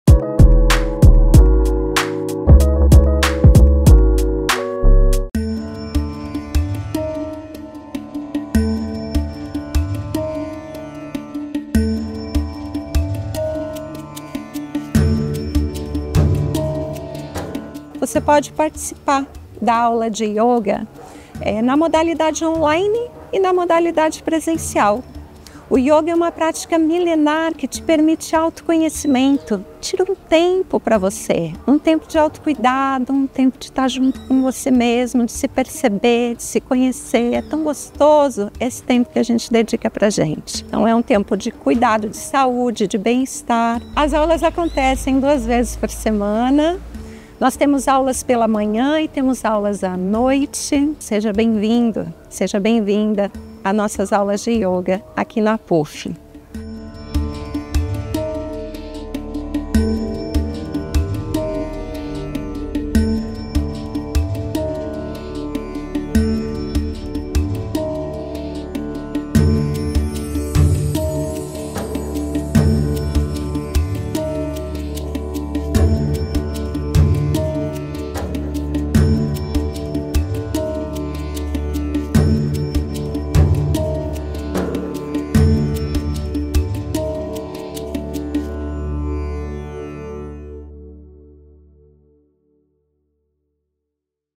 Aulas gratuitas de Yoga estão disponíveis aos docentes e aposentados filiados à APUFPR.
Aulas gratuitas de Yoga estão disponíveis aos docentes e aposentados filiados à APUFPR nas sedes de Curitiba e de Jandaia do Sul. ✨
🧘 Para quem não pode comparecer presencialmente, há a alternativa de horários de aulas on-line.